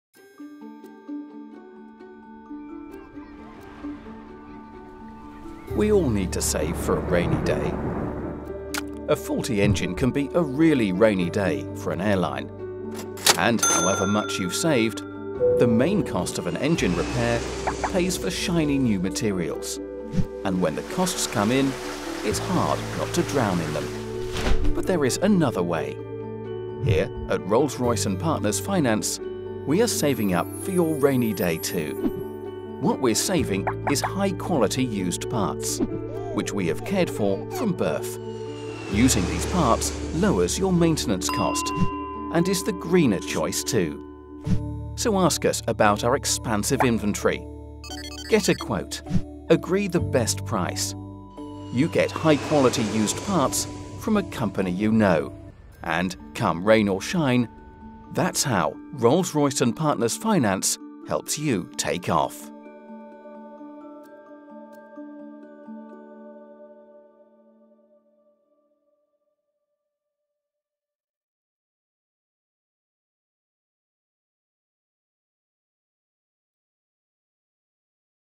Rolls-Royce | Used parts by Rolls-Royce & Partners Finance (RRPF)
Used parts are a great way to cut down on engine repair cost, and reduce the environmental impact of old engines.

Rolls-Royce & Partners Finance. Partnership is our business.

Rolls-Royce pioneers cutting-edge technologies that deliver clean, safe and competitive solutions to meet our planet’s vital power needs.